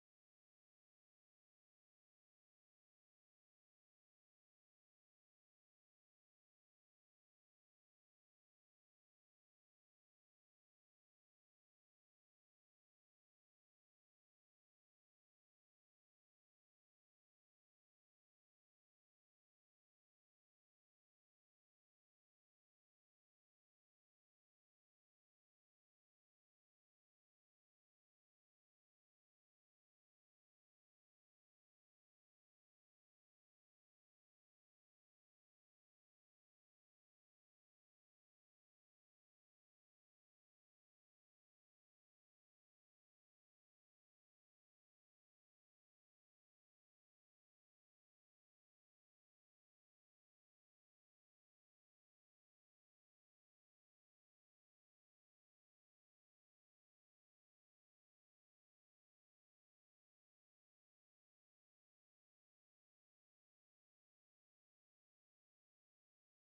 Elf Got Off a Shelf
Final project for a photoshop course. My first attempt at stop motion.